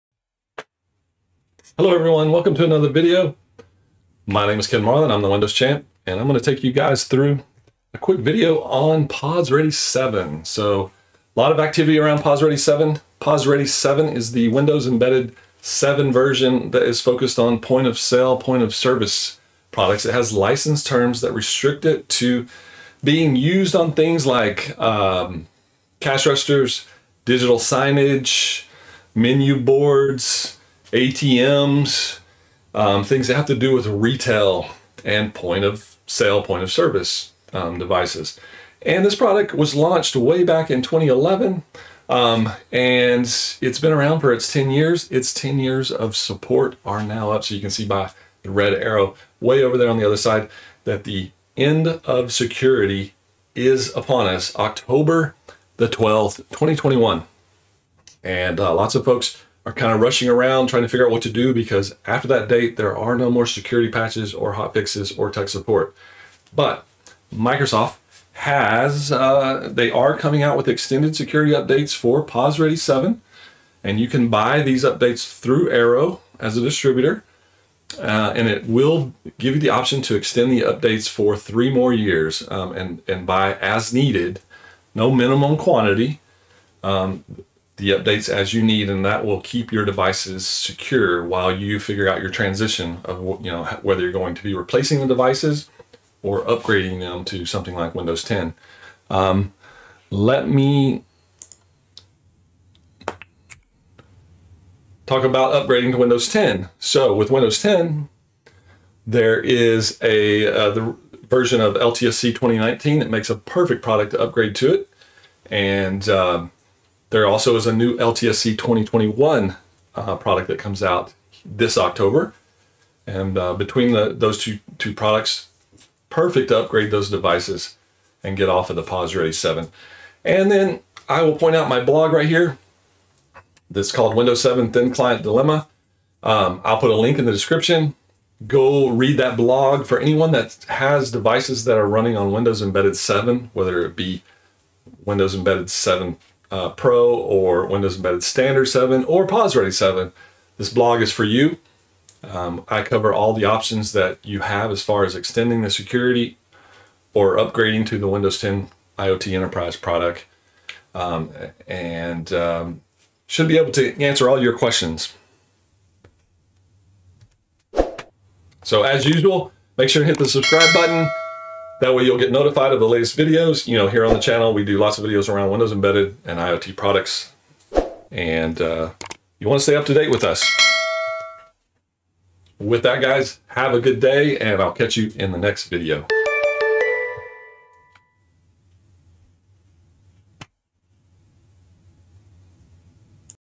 My Options for my POS Ready 7 Device
What are my options for my POSready 7 device when Microsoft ends support on 10/12/2021?  In this video, I cover the End of Support dates and how to extend the security updates or what options you have for moving to Windows 10 IoT Enterprise LTSC.